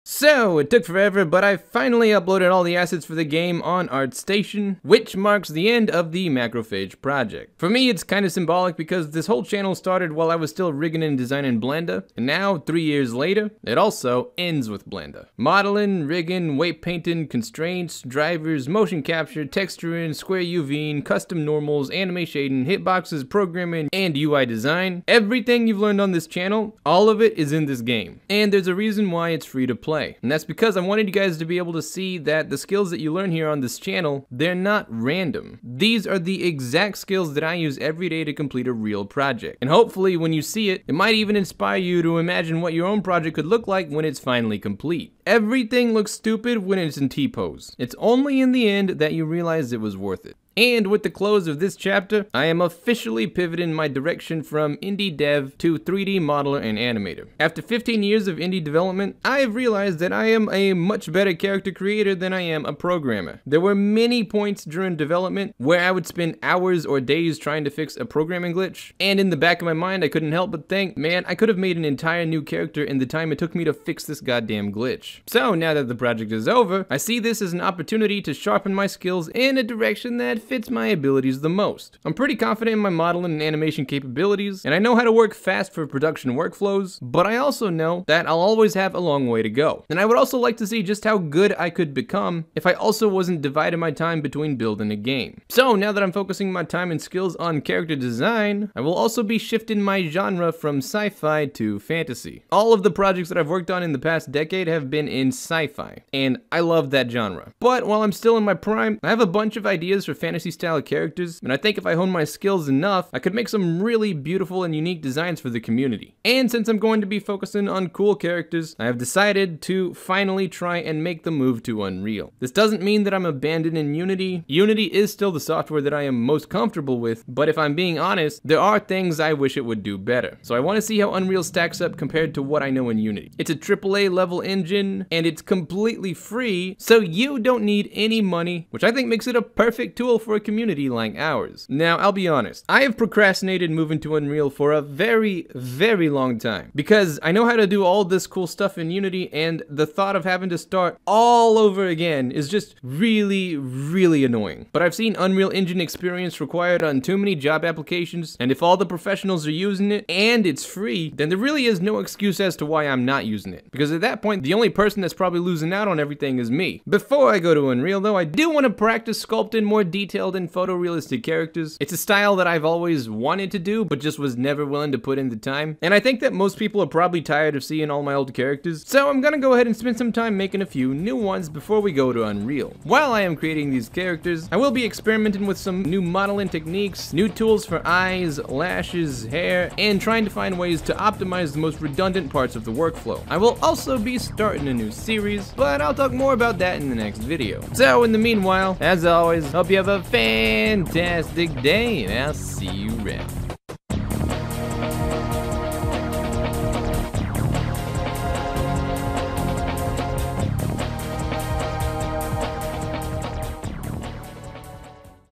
New Direction (What's NEXT!)
A lot of you were wondering what's going to happen now that the big project is over - So, today I'd like to share the direction I have for future plans going forward :)